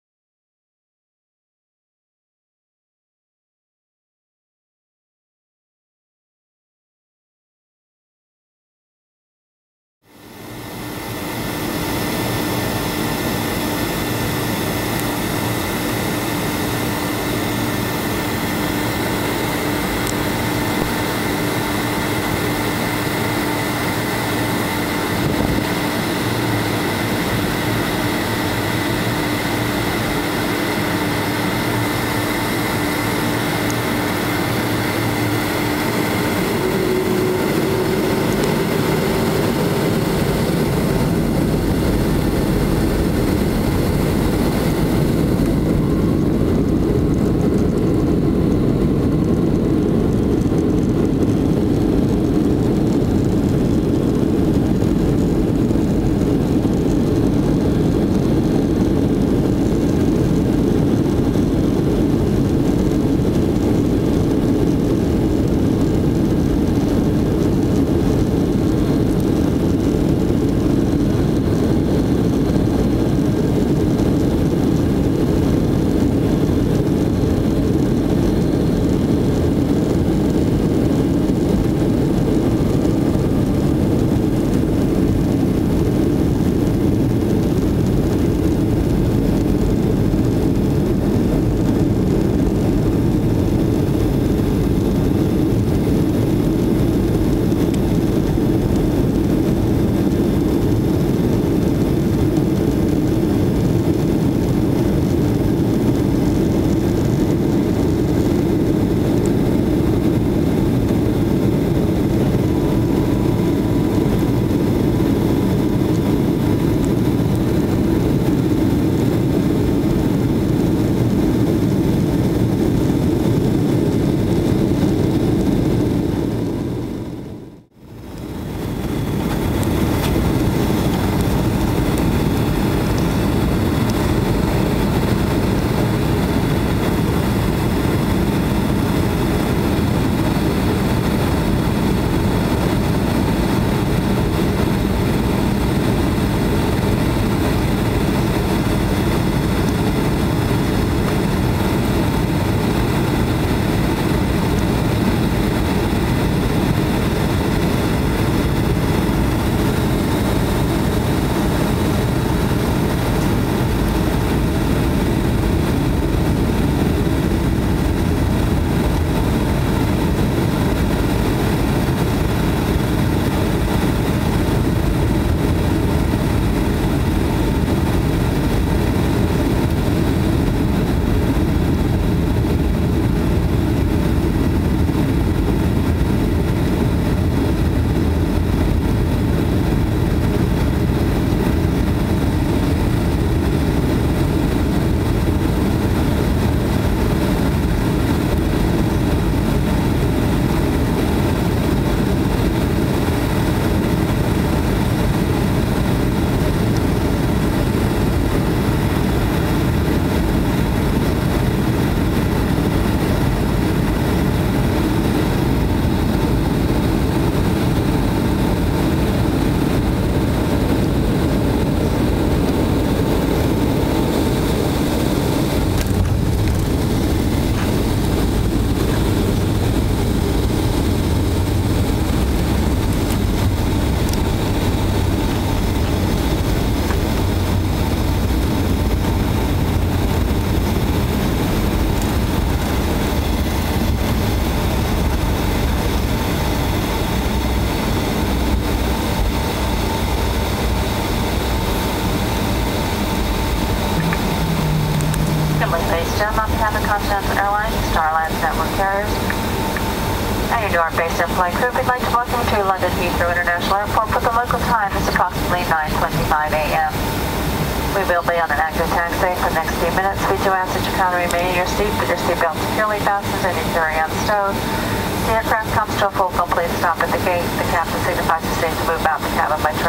Continental 757 Newark to London Heathrow (CO EWR-LHR)
Early 2011, nice little flight over the pond during the cold of winter in the states, but looks like early spring on the island. Trust me, it was still cold over there, humidity and cold do not mix nicely! Got my own row on the flight so had the chance to layout and sleep. Headed down to Brighton after we landed.  Landing in LHR is truly a unique experience, especially in the morning, circling in a holding pattern with a bunch of big jets is fun!  Notice all the 747s and the two 380s on landing.